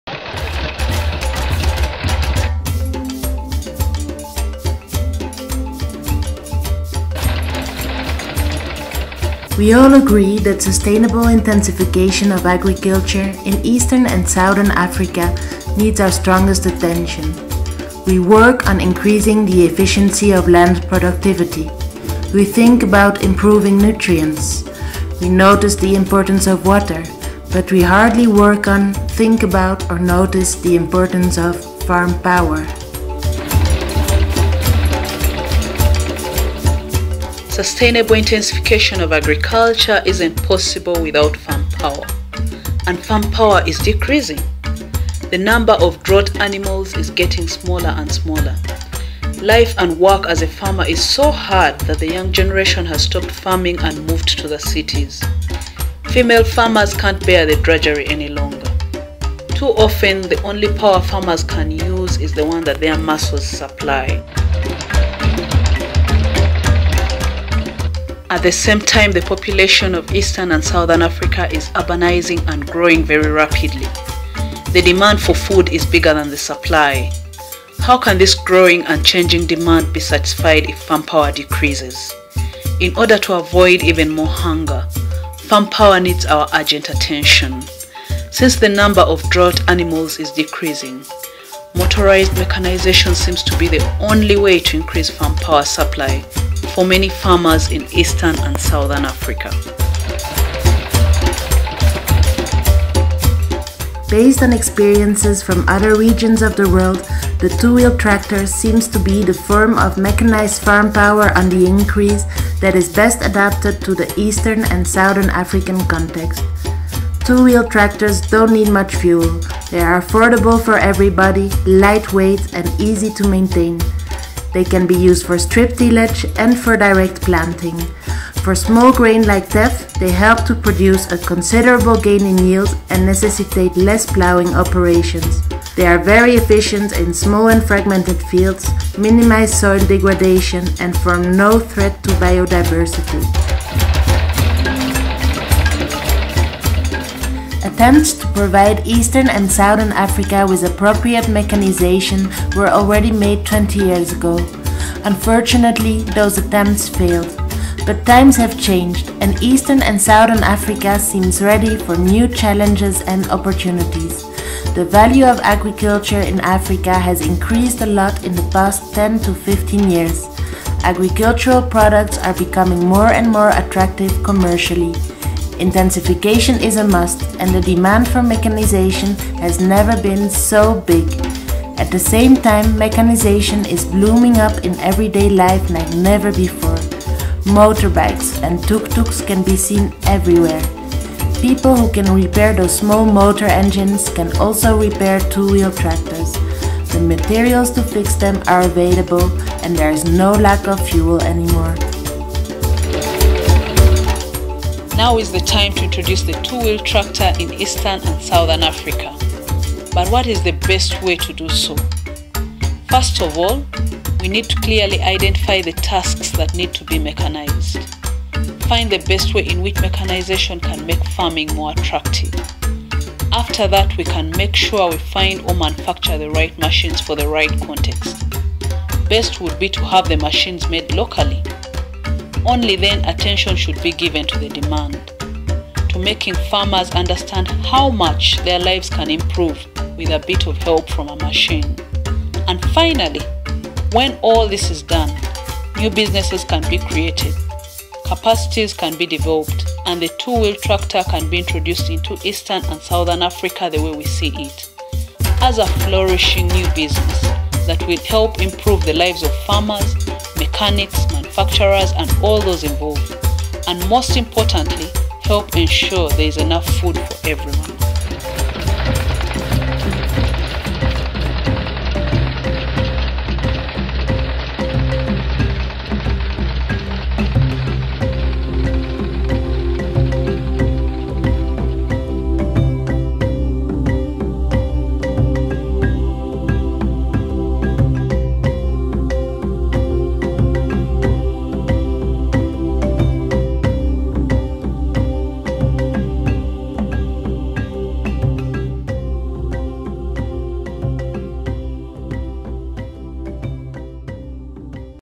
Powering Smallholder Agriculture in Eastern and Southern Africa
This digital story depicts the way in which 2-wheel tractors can offer a solution for the decrease in farm power in Eastern and Southern Africa. Commissioned and published by: The African Conservation Tillage Network (ACT) for the “Farm Mechanization and Conservation Agriculture for Sustainable Intensification” (FACASI) project (facasi.act-africa.org). FACASI is implemented by the International Maize and Wheat Improvement Center (CIMMYT) in Ethiopia, Kenya, Tanzania and Zimbabwe.